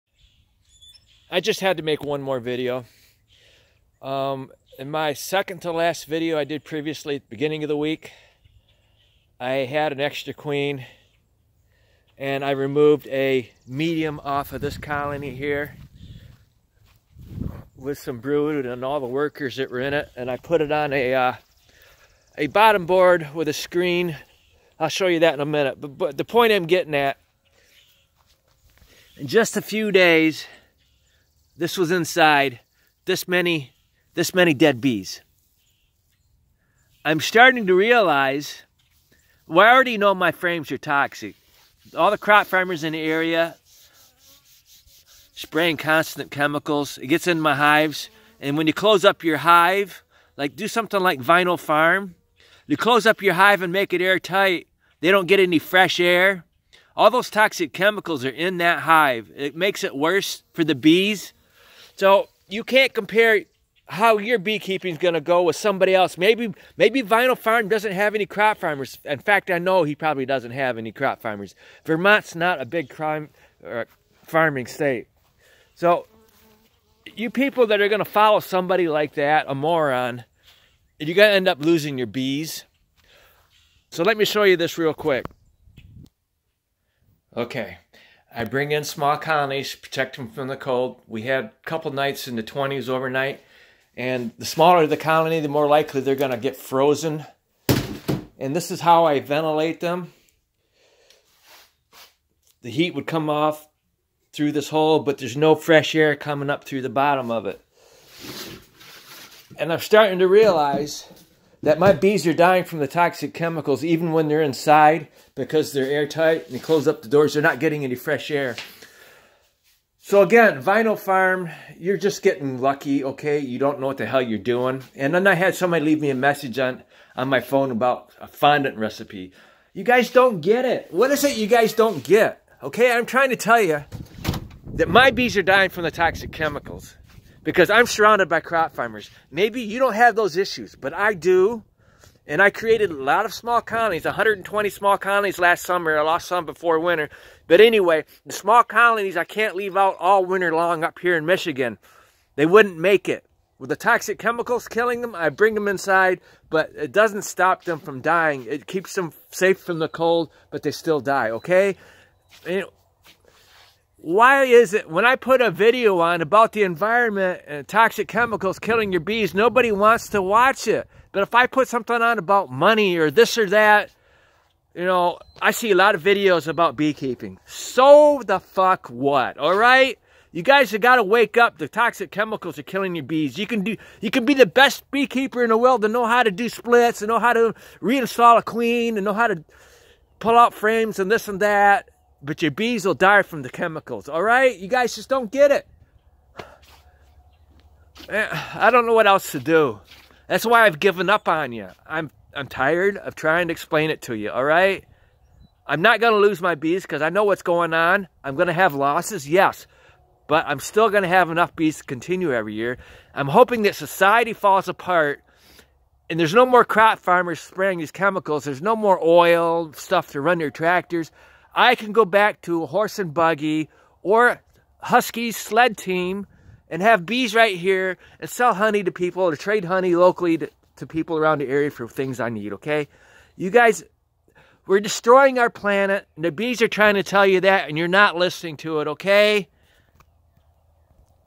One Last Video To Wake Up Beekeepers,U R Losing $!
Honey Bees do not live very long during winter because of the TOXIC CHEMICALS in their hive! You may survive a short cold spell but get wiped out during a long cold spell! Honey bee colonies will not increase size until there is food to gather. Feeding them syrup or other items is not the answer! We need to clean up our environment and allow honey bees to do what is natural.